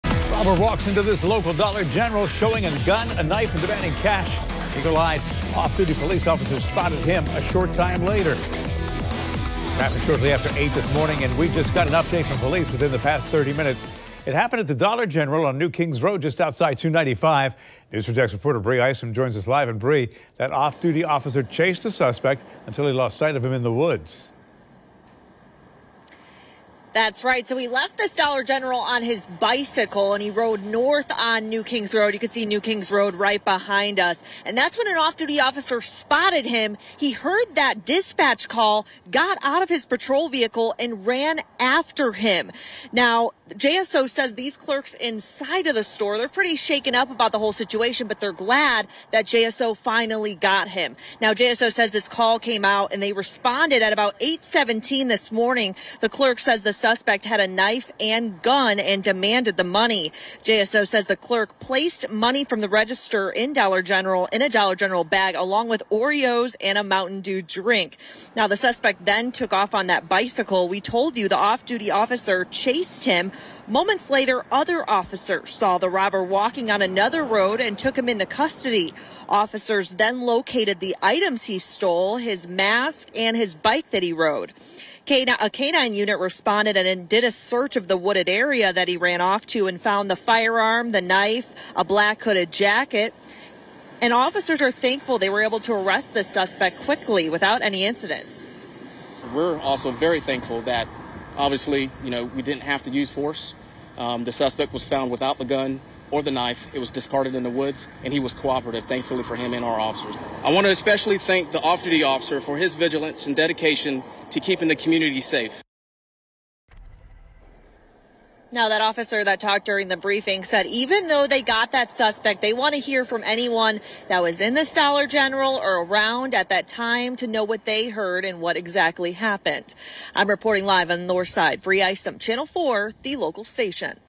Police locate person who took off from Dollar General with shopping bag filled with money, Oreos...
A person was taken into custody Monday morning after robbing a Dollar General store on the city’s Northside while armed and taking off with a shopping bag filled with money, cookies and a soft drink, according to the Jacksonville Sheriff’s Office.